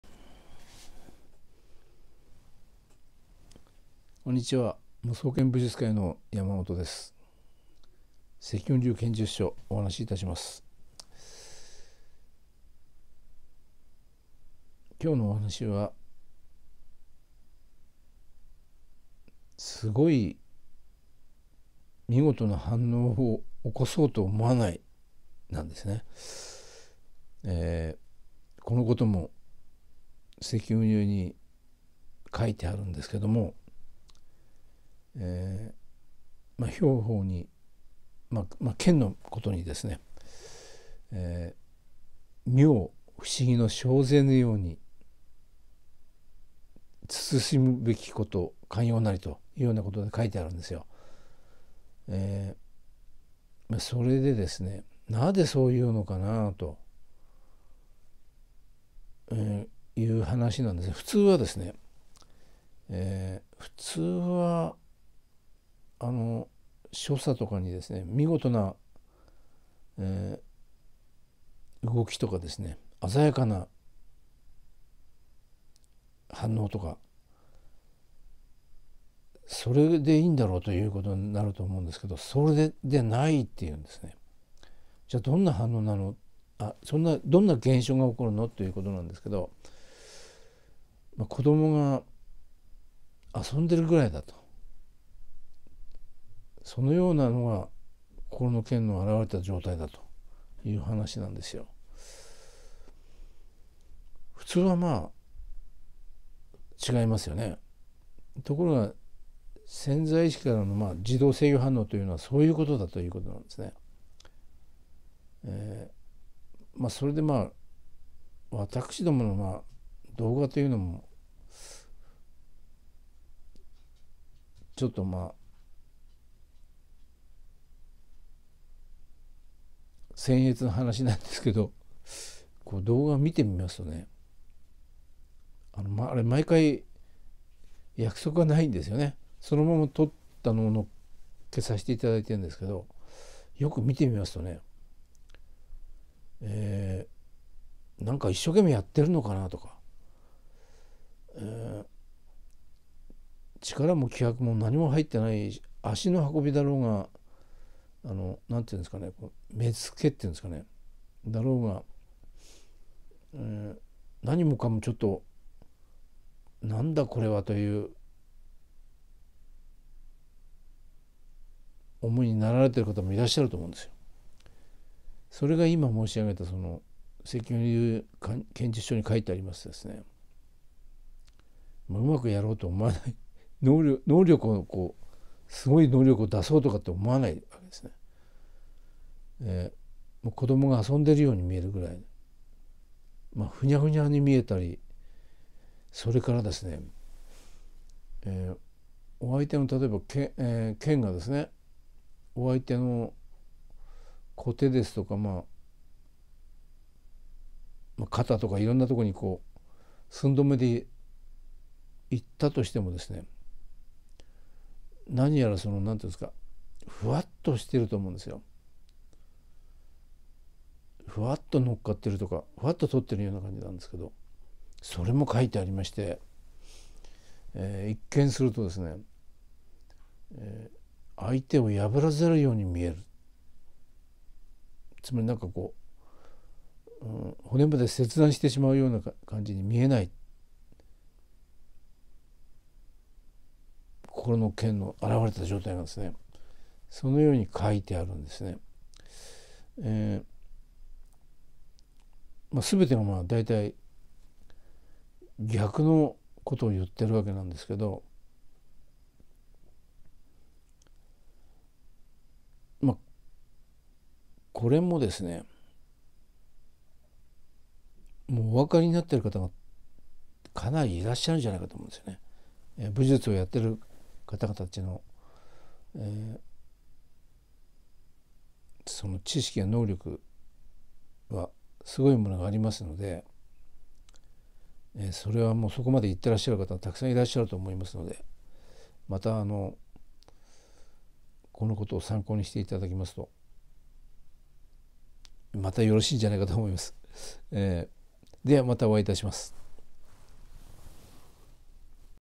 無想剣(３８７)見事に見えない様にする。
演武では無い、約束リハーサル無しの寸止め稽古でございます。
夕雲流や五輪ノ書など古伝の書には見事に見えない様にと記載されております。